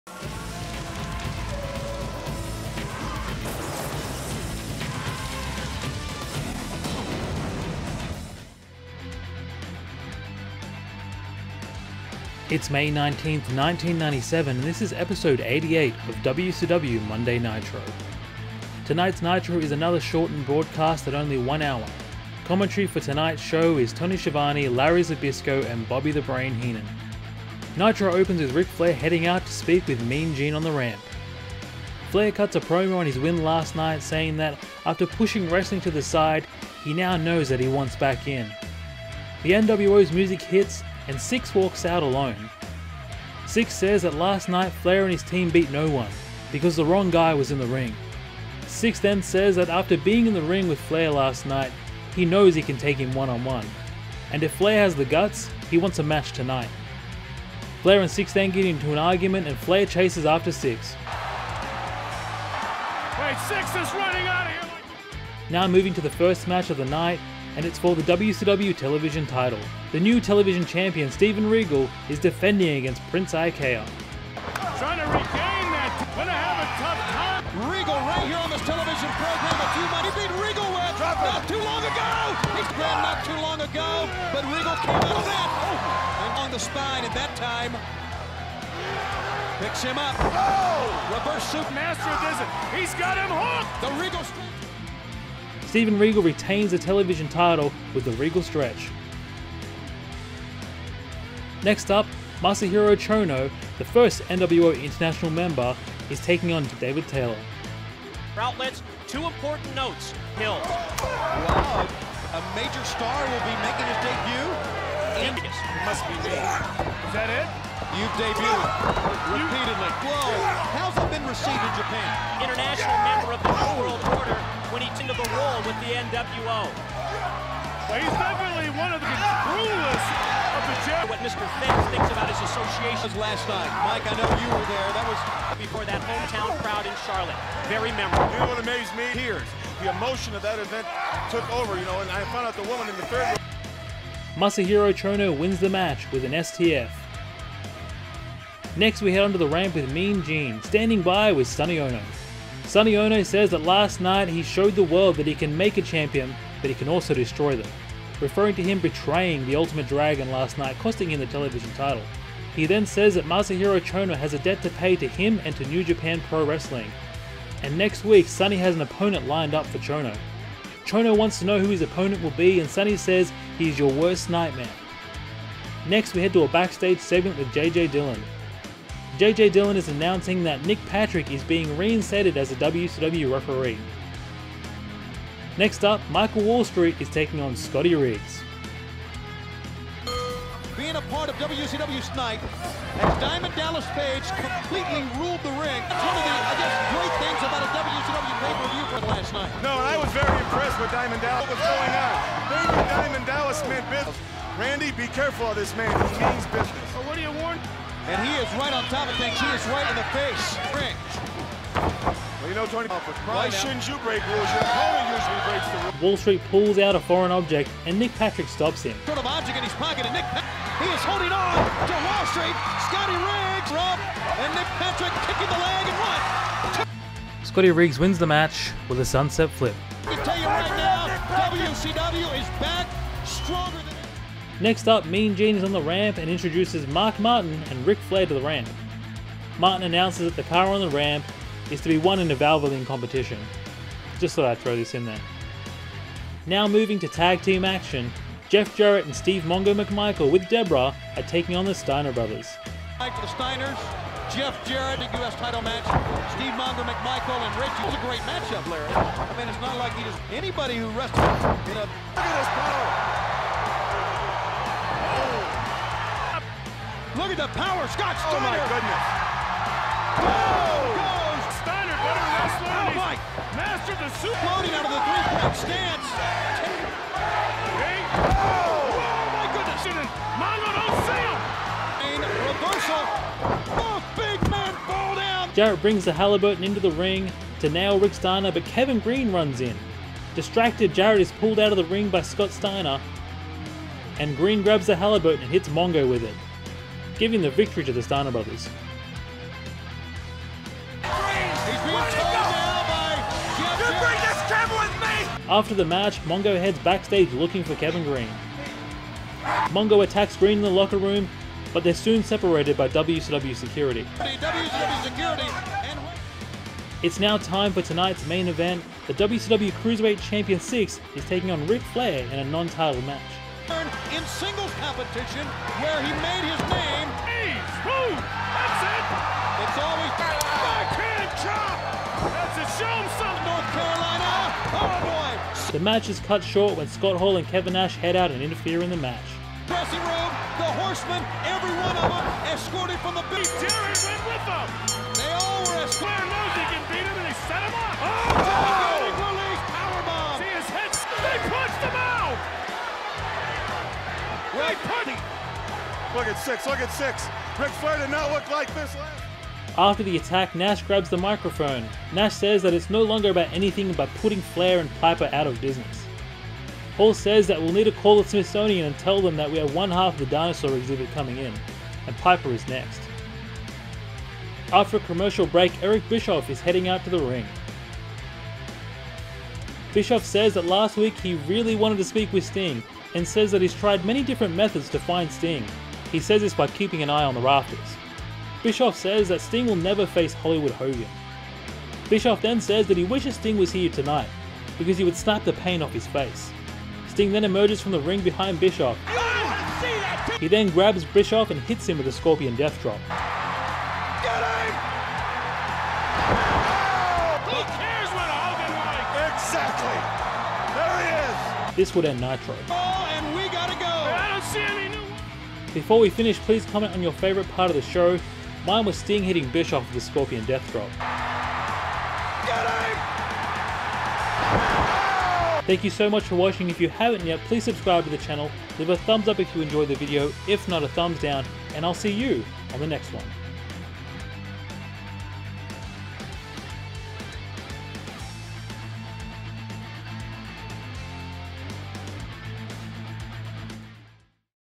WCW NITRO Recap Week 20 (1997) - Careful what you wish for...
WCW NITRO Recap Series.
Week 20: May 19th, 1997

0:00 Opening

07:18 My Favourite moment of NITRO

This recap series follows the WCW in 1997, most notably Monday Nitro..
I will also recap all the WCW PPVs for the Year.

*All events / shows discussed can be viewed in their entirety on the WWE Network. *